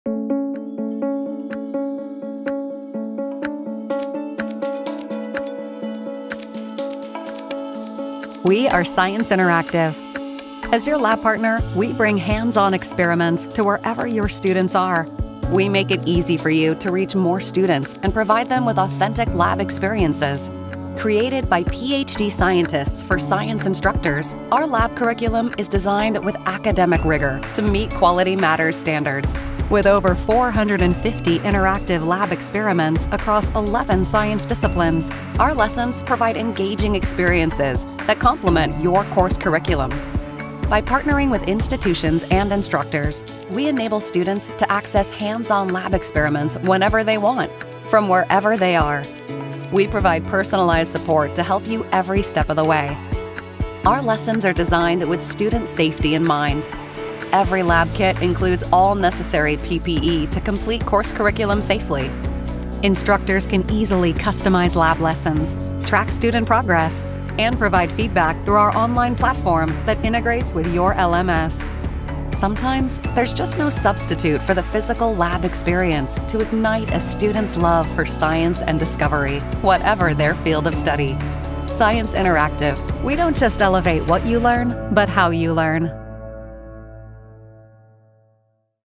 Science Interactive - Your Lab Partner 2023
As your lab partner, we bring hands-on and digital experiments to wherever your students are. We make it easy for your to reach more students and provide them with authentic lab experiences.

Created by PhD scientists for science instructors, our lab curriculum is designed with academic rigor to meet Quality Matters standards. With 450+ interactive lab experiments across 11 scientific disciplines, our lessons provide engaging experiences that complement your course curriculum.

Science Interactive: we elevate how students learn, not just what they learn.